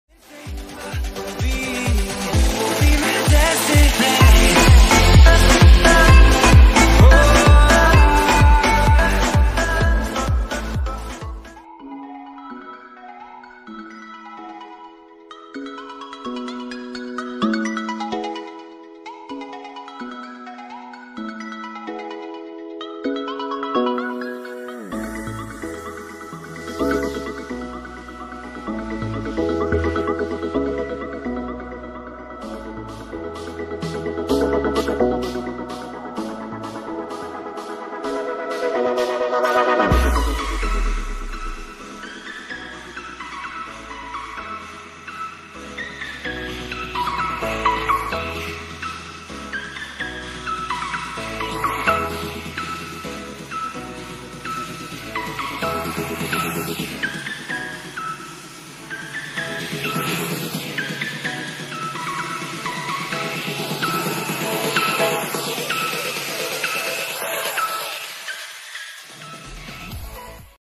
NEW PROMOCODE!!! (Neapolitan crown)
Is it a Domino Crown?? Is it a counterfeit?? Nope, its a crown... that you can EAT?!?!

NOTE: Music used is NOT owned by me!!